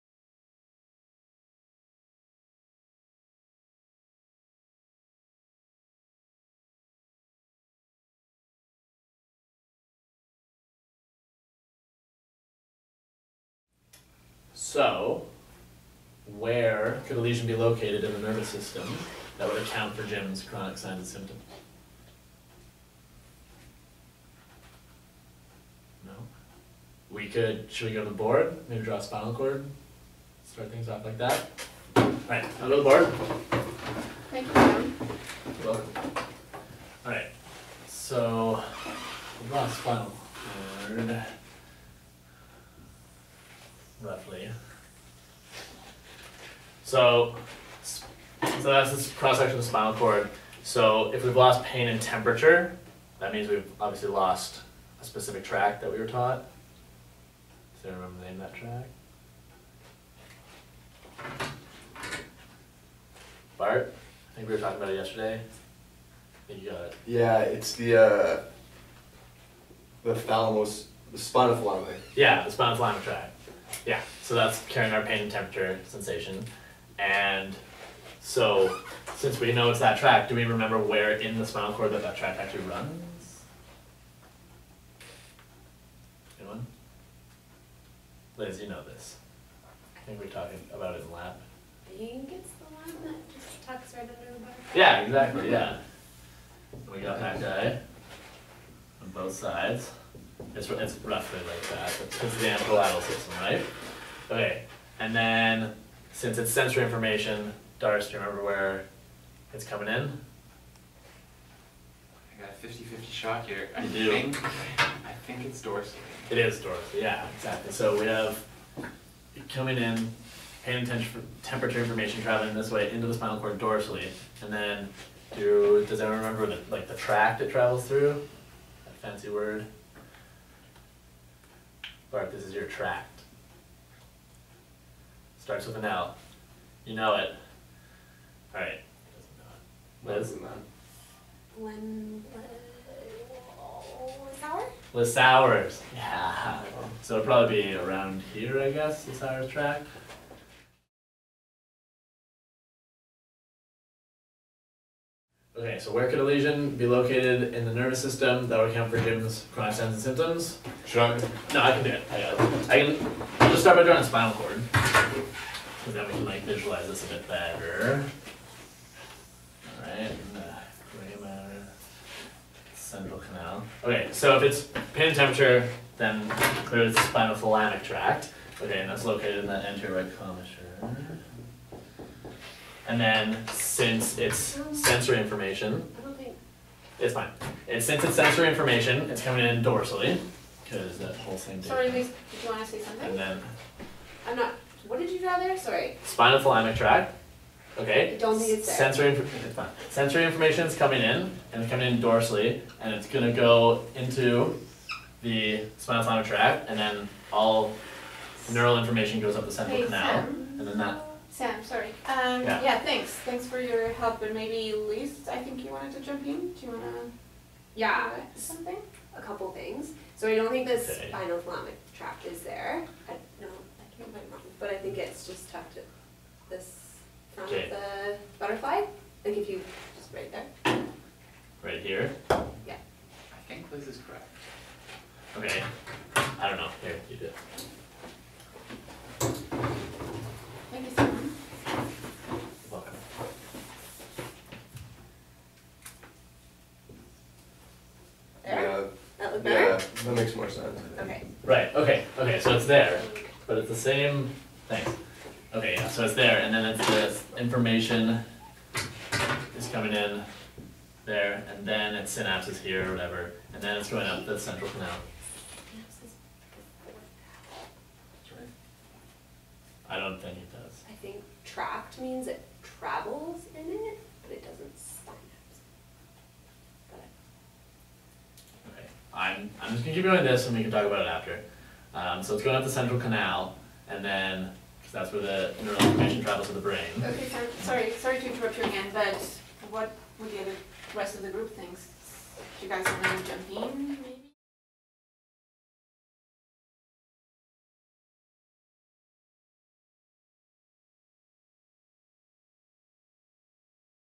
CBL Tutorial: The Dominating Student
CBL Tutor Training Scenarios: How to encourage positive contribution from a dominating student.

Created by UBC Medicine's Office of Faculty Development

Special thanks to the following faculty & students for making this video possible:

Dr. Jane Gair (director)
Dr. Joana Gil-Mohapel (CBL tutor)
Mr. Brett Giolma, Mr. Darcy Good, Ms. Elisabeth Pharo, Mr. Sam Brophy, and Ms. Tiffany Miller (Island Medical Program students).